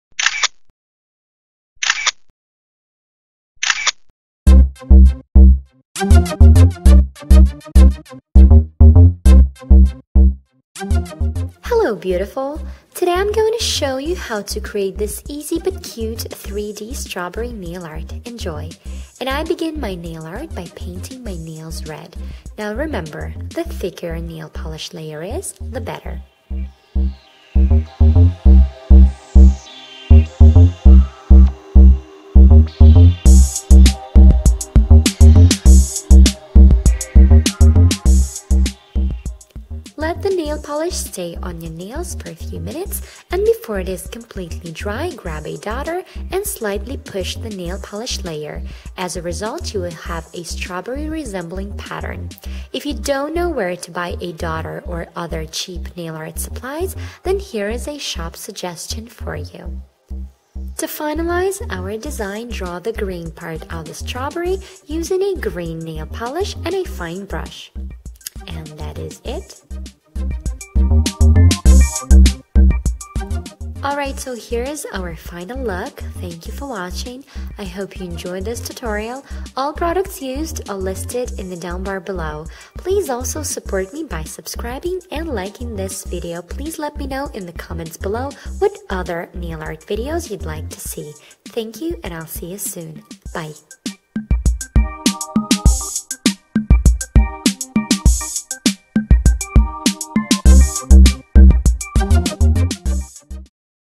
3D Strawberry Nail Art Tutorial
NOW I HAVE INSTAGRAM ACCOUNT! PLEASE FOLLOW!

Used products / Использованные продукты:
Isadora Wonder Nail Wide Brush Nail Polish in 644 Red Alert
Nail Paint by BarryM in 290 Spring Green
Dotter